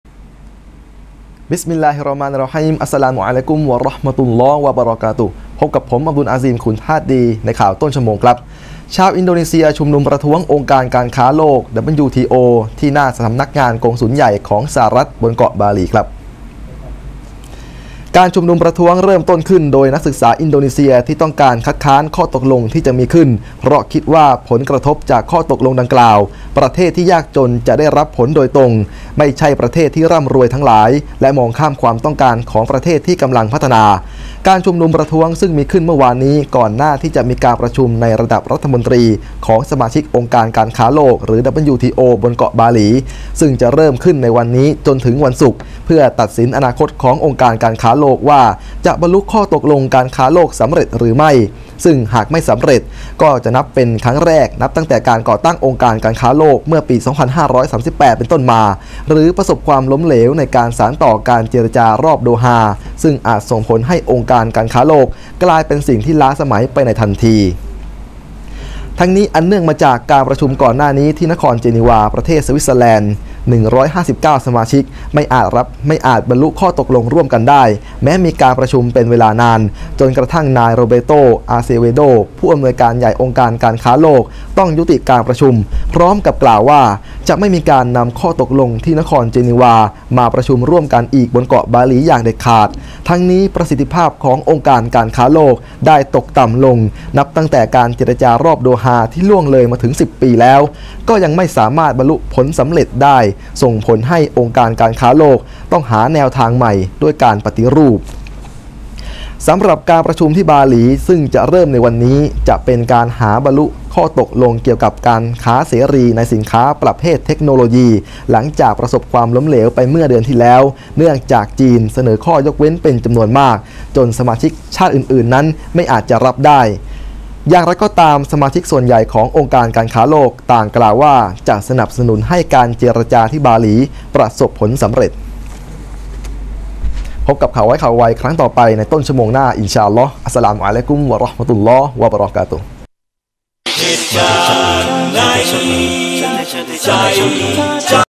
ชาวอินโดนีเซียชุมนุมประท้วง WTO
การชุมนุมประท้วงเริ่มต้นขึ้น โดยนักศึกษาอินโดนีเซียที่ต้องการคัดค้านข้อตกลงที่จะมีขึ้น เพราะคิดว่าผลกระทบจากข้อตกลงดังกล่าว ประเทศที่ยากจนจะได้รับผลโดยตรง ไม่ใช่ประเทศที่ร่ำรวยทั้งหลาย และมองข้ามความต้องการของประเทศที่กำลังพัฒนา การชุมนุมประท้วง ซึ่งมีขึ้นเมื่อวานนี้ ก่อนหน้าที่จะมีการประชุมในระดับรัฐมนตรี ของสมาชิกองค์การการค้าโลก หรือ WTO  บนเกาะบาหลี ซึ่งจะเริ่มขึ้นในวันนี้จนถึงวันศุกร์ เพื่อตัดสินอนาคตขององค์การการค้าโลก ว่าจะบรรลุข้อตกลงการค้าโลกสำเร็จหรือไม่ ซึ่งหากสำเร็จก็จะเป็นครั้งแรกนับแต่ก่อตั้งองค์การการค้าโลก เมื่อปี 2538 เป็นต้นมา หรือประสบความล้มเหลวในการสานต่อการเจรจารอบโดฮา ซึ่งอาจส่งผลให้องค์การการค้าโลกกลายเป็นสิ่งล้าสมัยไปในทันที
             ทั้งนี้ อันเนื่องมาจากการประชุมก่อนหน้านี้ ที่นครเจนีวา ประเทศสวิตเซอร์แลนด์ 159 ชาติสมาชิกไม่อาจบรรลุข้อตกลงร่วมกันได้ แม้มีการประชุมเป็นเวลานาน จนกระทั่งนายโรแบร์โต อาเซเวโด ผู้อำนวยการใหญ่องค์การการค้าโลก ต้องยุติการประชุม พร้อมกับกล่าวว่า จะไม่มีการนำข้อตกลงที่นครเจนีวา มาประชุมร่วมกันอีกที่เกาะบาหลีอย่างเด็ดขาด ทั้งนี้ ประสิทธิภาพขององค์การการค้าโลก ได้ตกต่ำลง นับแต่การเจรจารอบโดฮาที่ล่วงเลยมาถึง 10 ปีแล้ว ก็ยังไม่สามารถบรรลุผลสำเร็จได้ ส่งผลให้องค์การการค้าโลก ต้องหาแนวทางใหม่ด้วยการปฏิรูป
            สำหรับการประชุมที่บาหลี ซึ่งจะเริ่มในวันนี้ จะเป็นการหาทางบรรลุข้อตกลงเกี่ยวกับการค้าเสรีในสินค้าประเภทเทคโนโลยี หลังจากประสบความล้มเหลวไปเมื่อเดือนที่แล้ว เนื่องจากจีนเสนอข้อยกเว้นเป็นจำนวนมาก จนสมาชิกชาติอื่นๆ ไม่อาจรับได้ อย่างไรก็ตาม สมาชิกส่วนใหญ่ขององค์การการค้าโลก ต่างกล่าวว่าจะสนับสนุนให้การเจรจาที่บาหลีประสบผลสำเร็จ